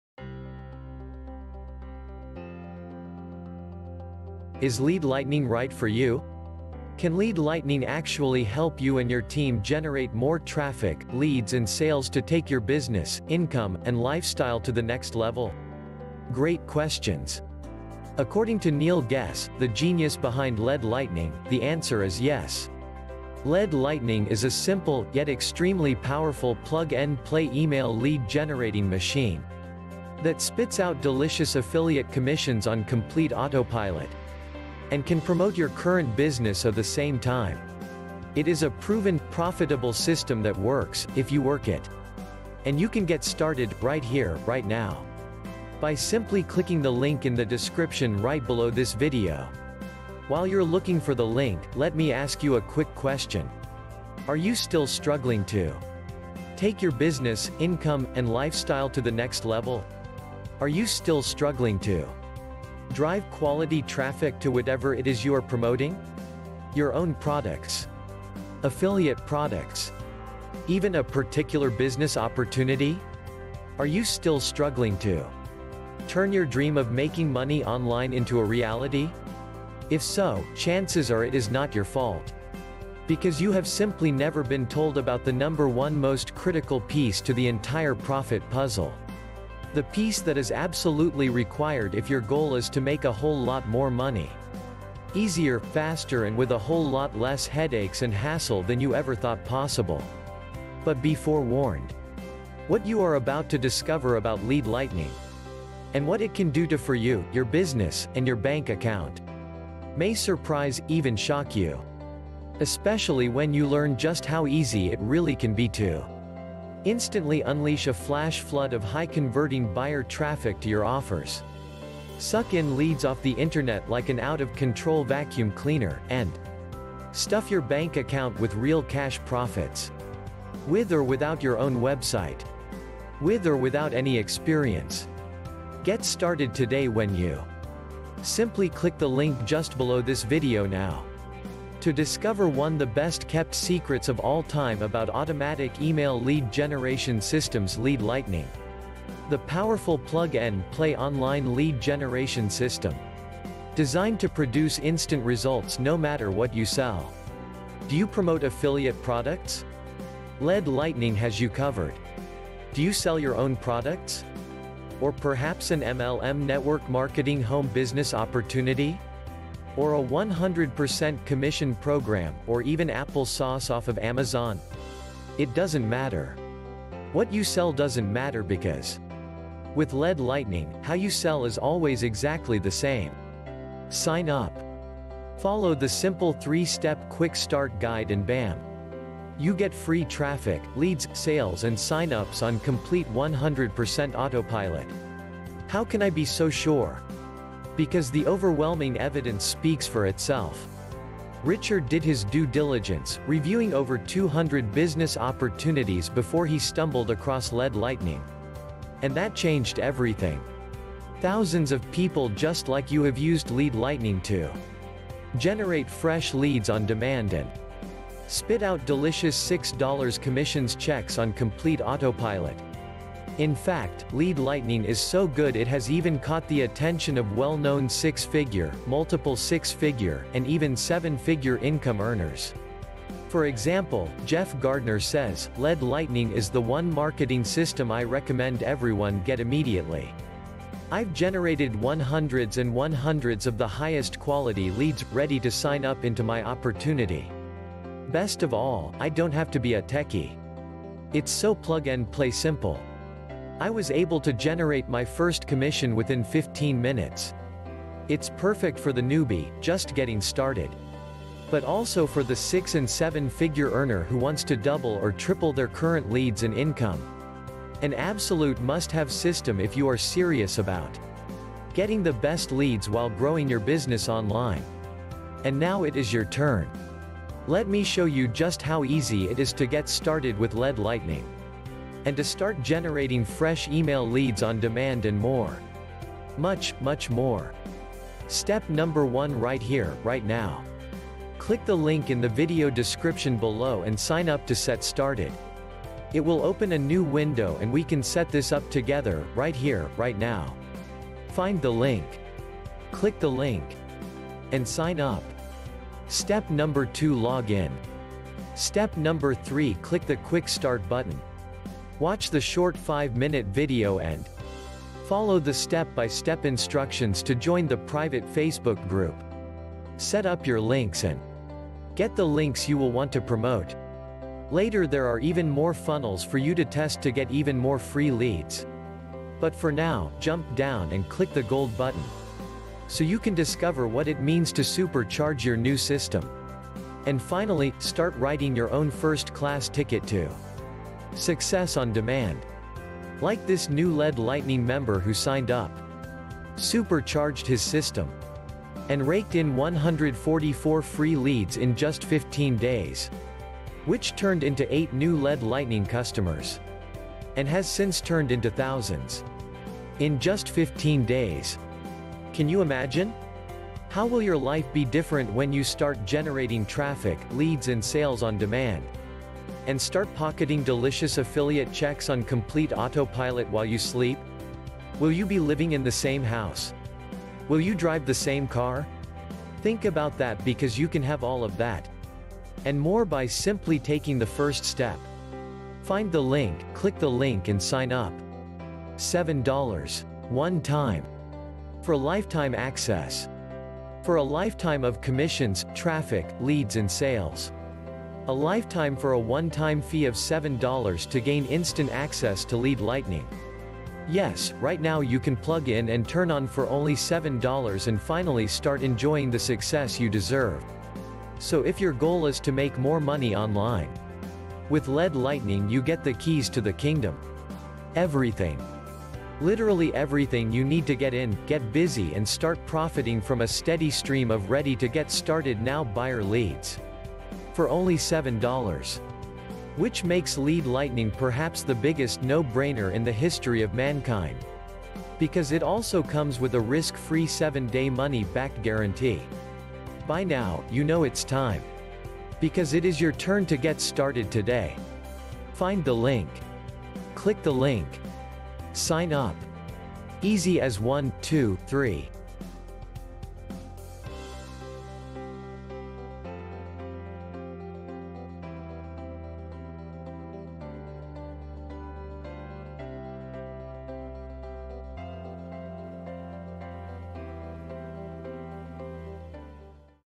Lead Lightening - Is the Lead Lightning Marketing System Right for You
Time Stamps
0:00 - Is Lead Lightning Right for You?
0:28 - Plug-and-play email lead generation
0:43 - How to get started
1:29 - Solving the profit puzzle
2:27 - Automatic email lead generation systems
3:00 - What you sell doesn’t matter
3:22 - Due Diligence
4:23 - Perfect for newbies
5:28 - More funnels
6:25 - The first step
7:18 - The Guarantee

Can Power Lead System’s Lead Lightning system actually help you and your team generate more traffic, leads and sales to take your business, your income, and your lifestyle to the next level?

The answer is YES! The Lead Lightning marketing system is a simple, yet extremely powerful plug-and-play email lead generating machine that also spits out delicious affiliate commissions on complete autopilot while promoting your current business at the same time.

And, the associated compensation plan can’t be beat. For a one-time investment of only $7, you could crank out a virtually unlimited flow of $6 commission checks. And, the folks at Power Lead System will show you how you can supercharge the system for even higher commissions.

We all know that leads are the lifeblood of any business, and the Lead Lightning marketing funnel is designed to create a constant flow of leads for your business. Any business.

Online lead generation has never been easier. Just sign up, follow a simple 3-step quick start guide, and you’re on your way to free traffic, leads, and sales on complete autopilot.

And you don’t even need your own website, hosting account, or autoresponder to make it happen. You don’t even need any online marketing experience to make this system work. It’s 100% newbie-friendly.

Lead Lightning is the second of the six products offered by the Power Lead System. The product line consists of:

Free Lead System
Lead Lightning
Power Lead System
Free Ads Secrets
Social Profit Academy
Master Traffic Institute

all of which are designed to help your online lead generation supported by all the tools you need to generate those leads and turn them into sales.

to find out more about these amazing resources. I have created a playlist specifically for videos relating to the Power Lead System